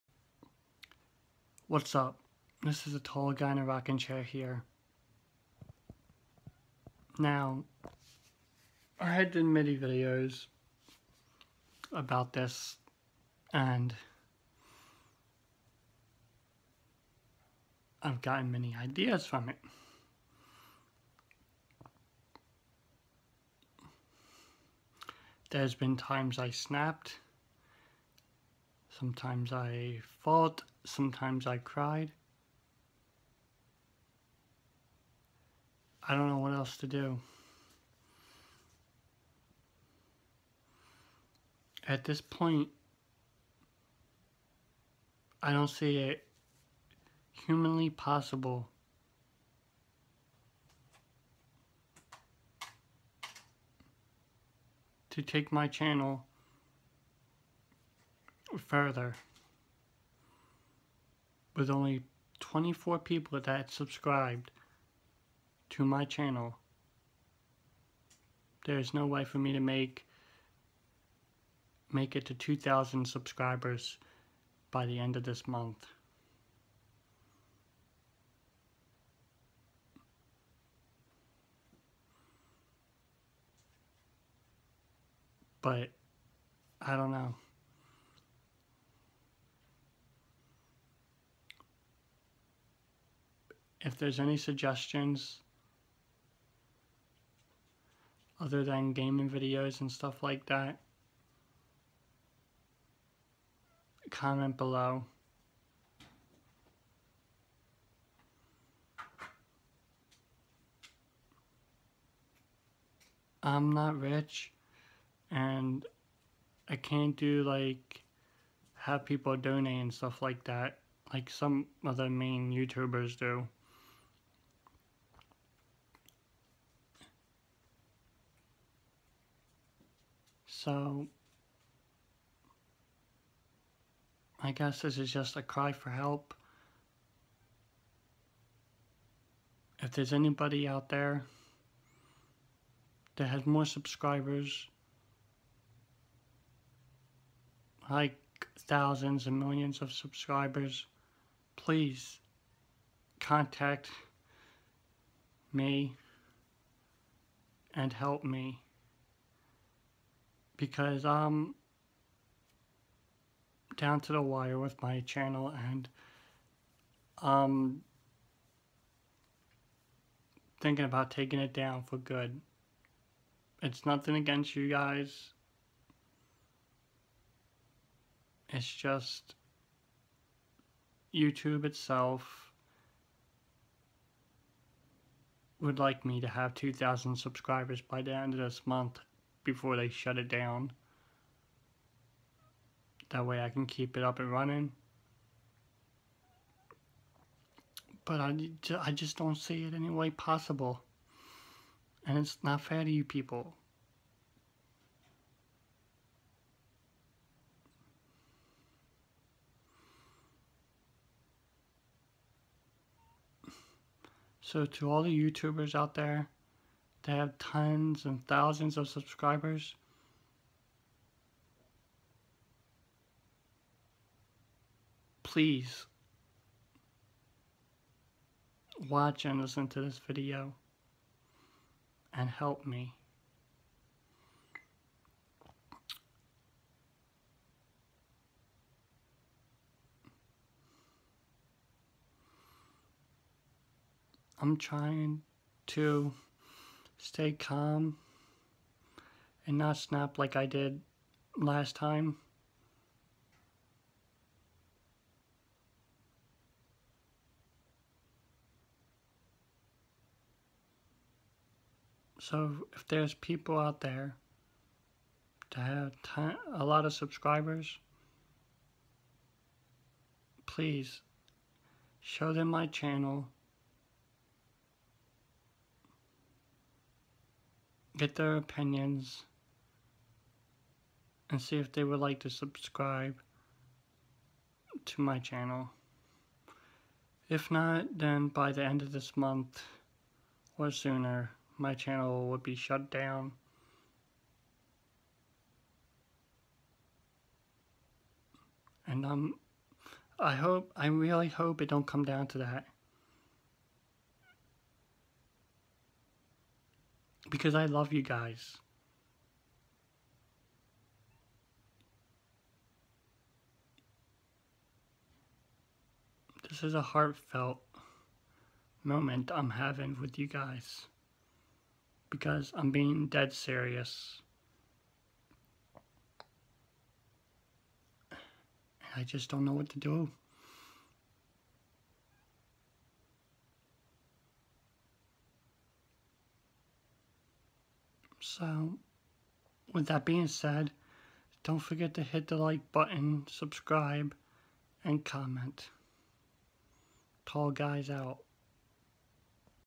Tall guy has a heart felt speech
Tall guy speaks from the heart and have a heart felt moment and ask for everyone to help him!!!